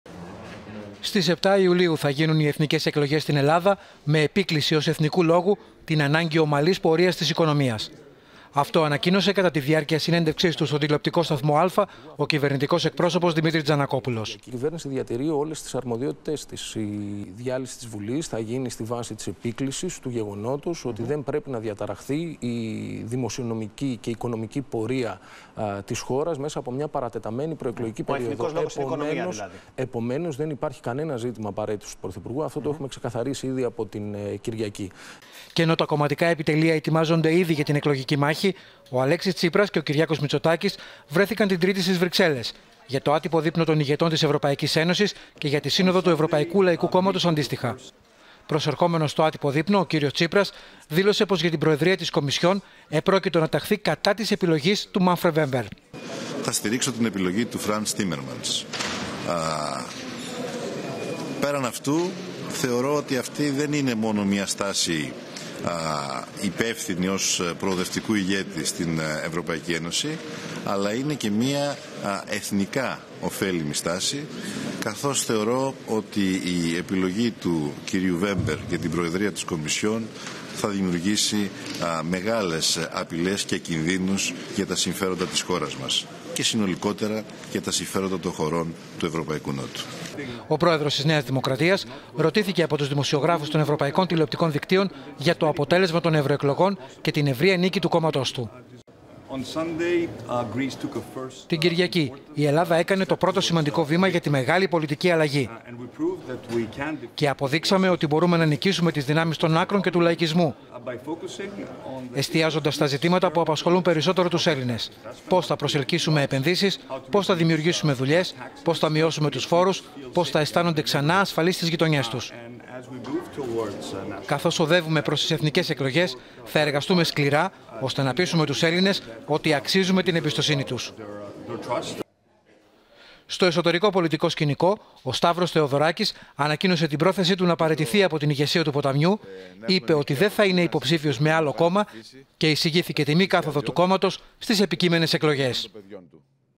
Ελλάδα: Στις 7 Ιουλίου οι εκλογές
Η απόφαση του πρωθυπουργού είναι πράγματι, οι εκλογές να γίνουν στις 7 Ιουλίου για να μην διαταραχθεί η διαδικασία των (σχολικών) εξετάσεων», τόνισε ο κυβερνητικός εκπρόσωπος…